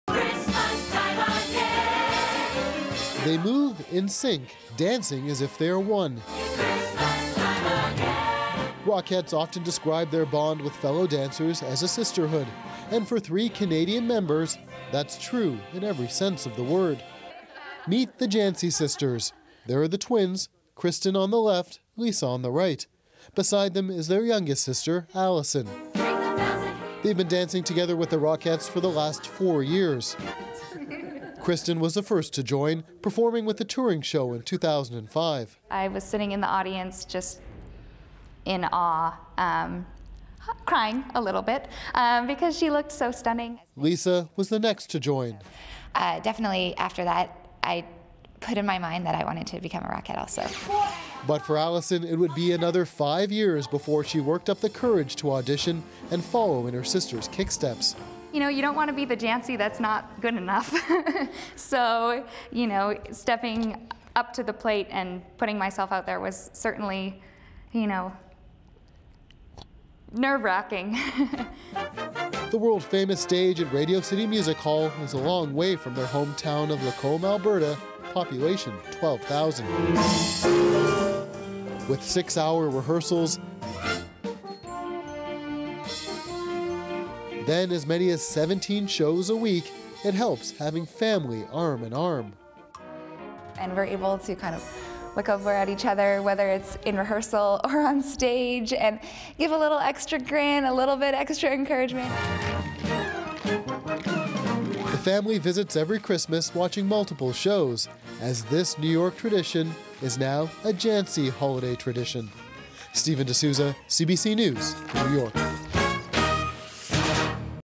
Canadian sisters make Rockettes a family affair
Three sisters from small-town Alberta are a main attraction at New York's Radio City Christmas Spectacular.